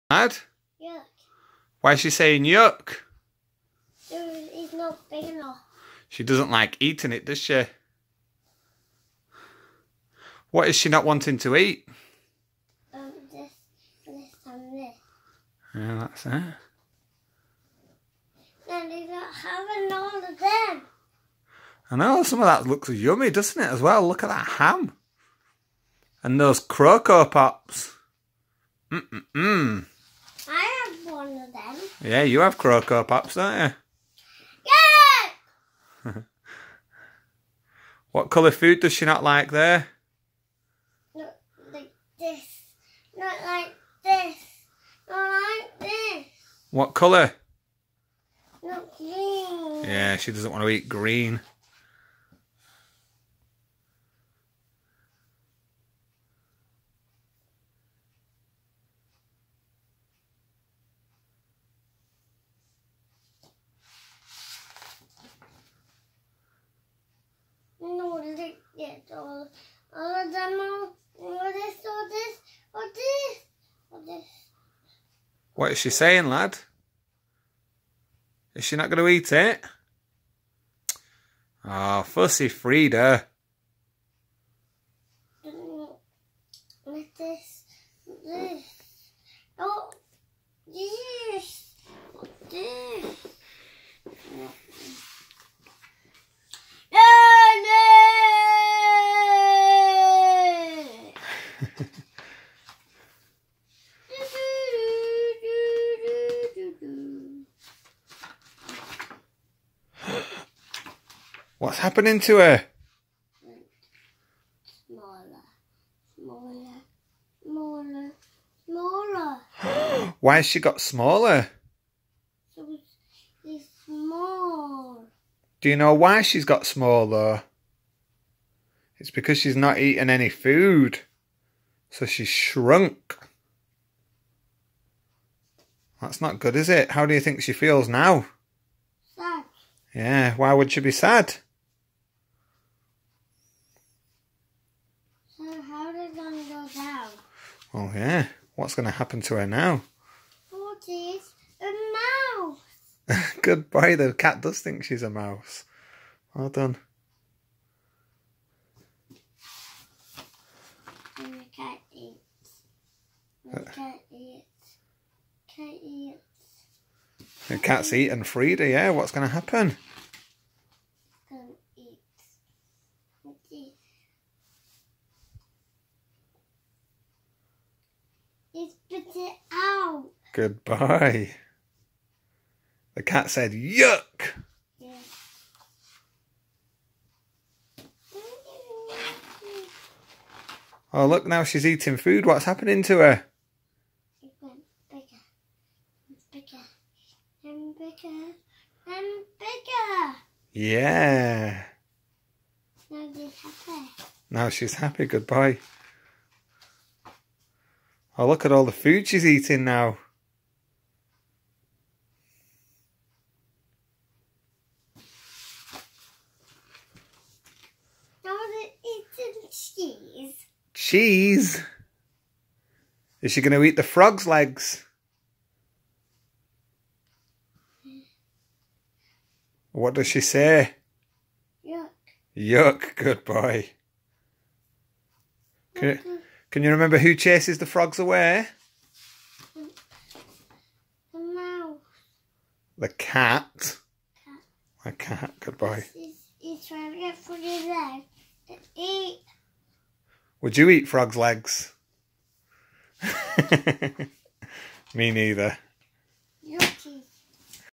T reads Fussy Freda by Julia Jarman and Fred Blunt!
A problem all parents will face at some point!